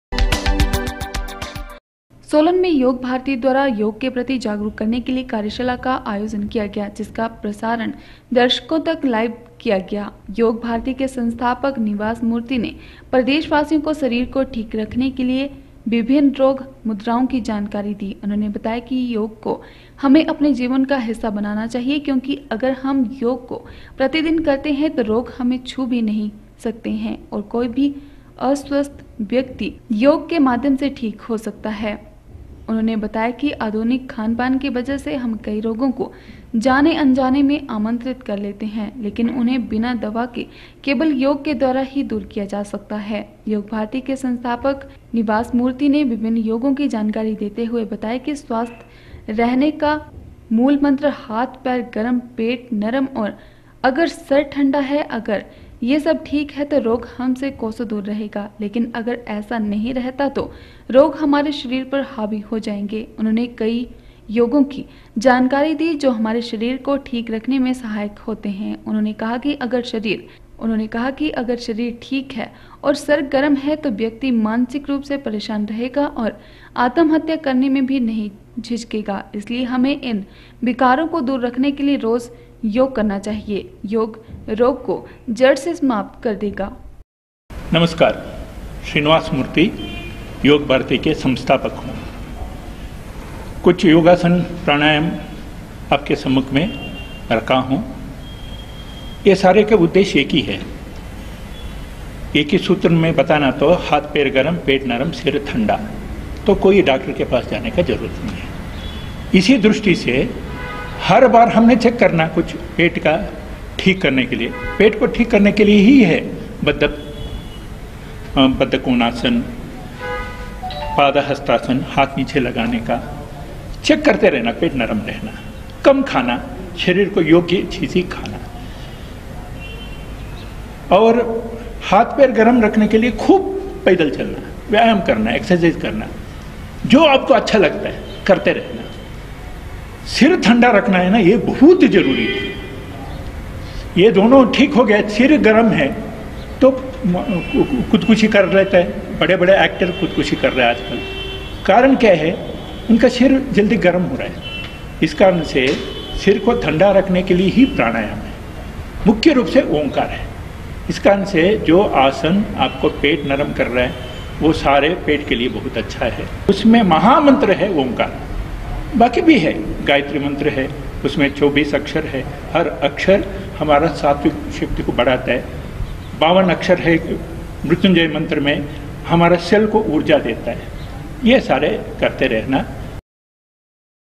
17 june 12 स्वास्थ्य रहने का मूल मंत्र हाथ पैर गर्म पेट नरम और सर ठंडा
स्वास्थ्य रहने का मूल मंत्र हाथ पैर गर्म पेट नरम और  सर ठंडा  :  योग भारती  संस्थापक श्री निवास मूर्ती
  सर गर्म रहने पर ही लोग आत्महत्या करने पर हो जाते हैं मजबूर